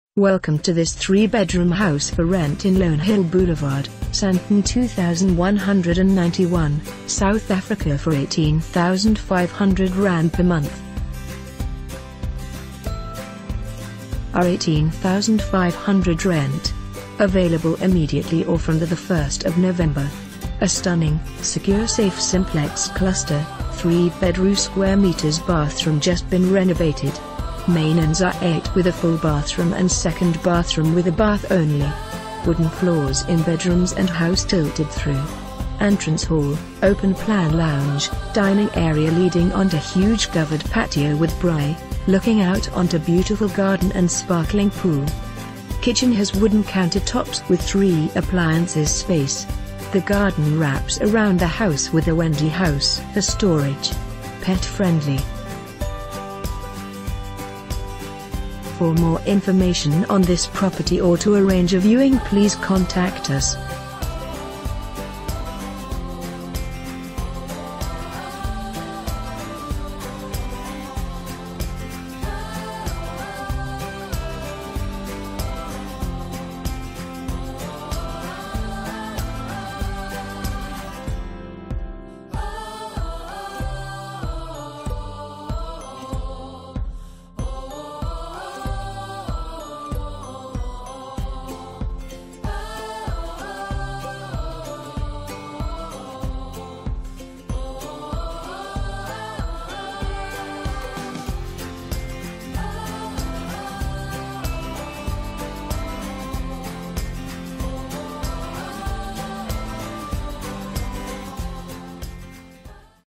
3 Bedroom House For Rent in Lonehill Boulevard, Sandton 2191, South Africa for ZAR 18,500 per month
To view videos of similar listings in Lonehill, Sandton, Gauteng please

Property Description

R18500 rent
Available immediately or from the 1 November
A stunning , secure safe simplex cluster , 3 bedroom 2 bathroom just been renovated .main ens-uite with a full bathroom and second bathroom with a bath only. Wooden floors in bedrooms and house tilted through
Entrance hall ,open plan lounge , dining area leading onto...